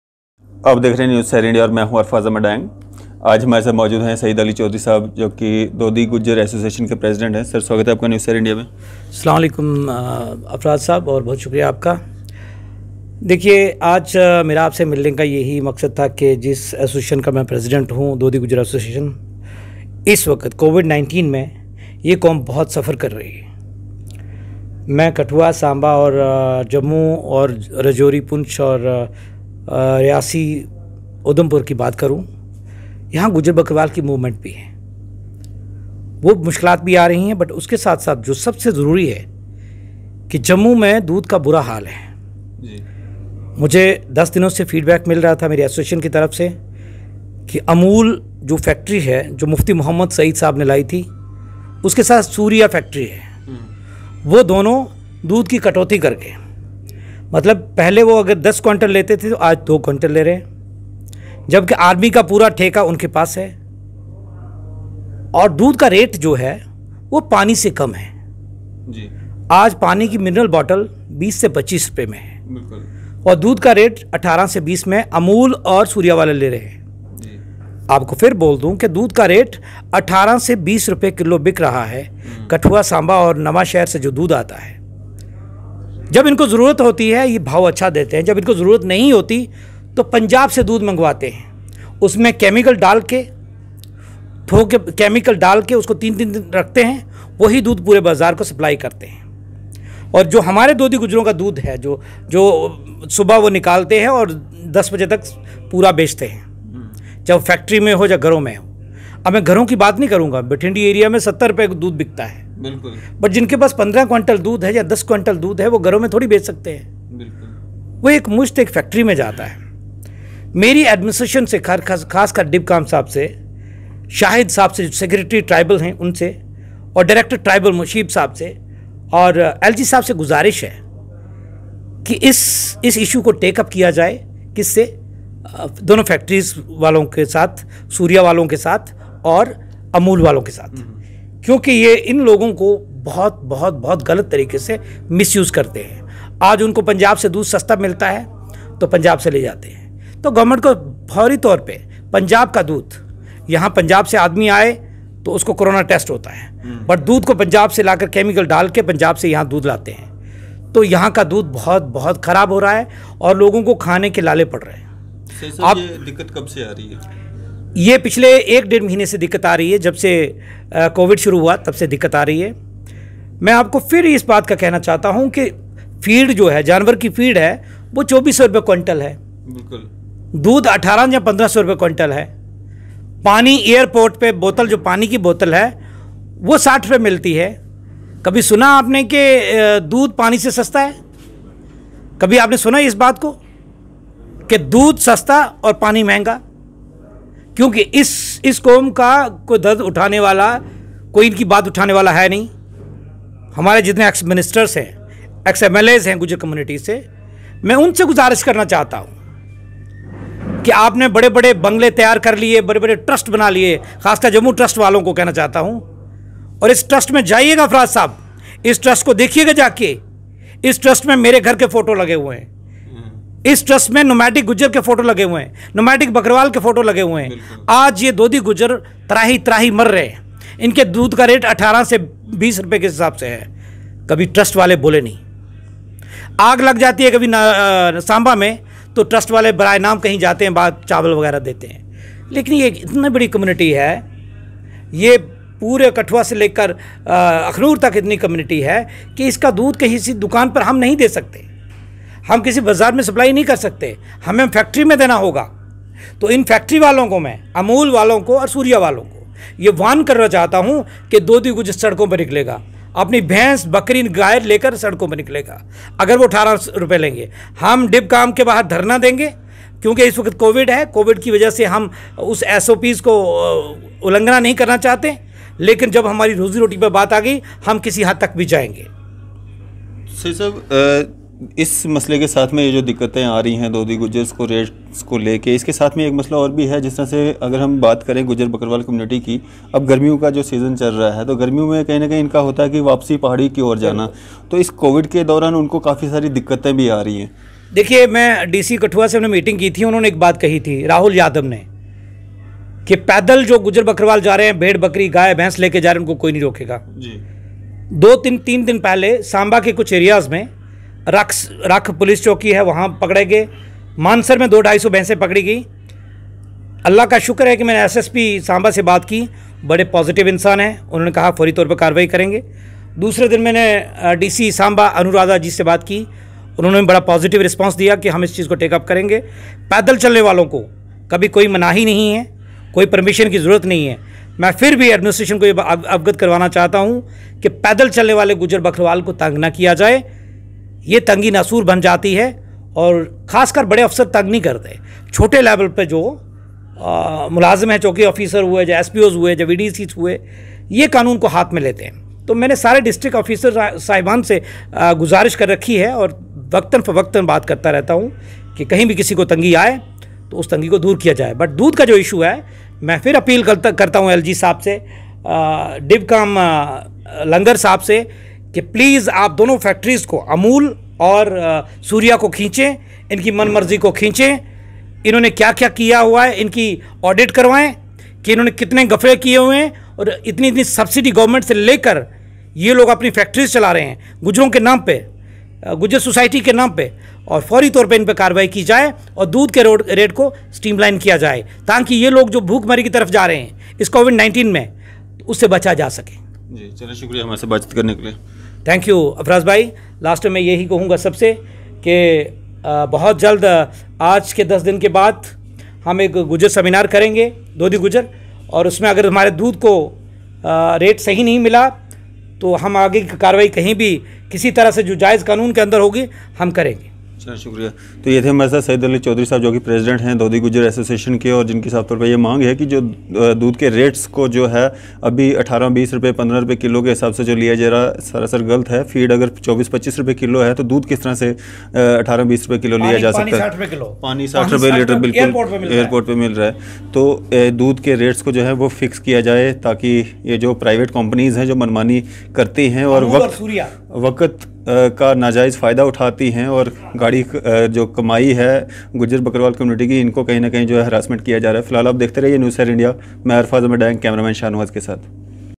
Lockdown k Doraan Gujjaron k sath ki jaarehe hai nainsafi....President Dhodi Gujjar Association Syed
Lockdown k Doraan Gujjaron k sath ki jaarehe hai nainsafi. President Dhodi Gujjar Association Syed Alli Choudhry.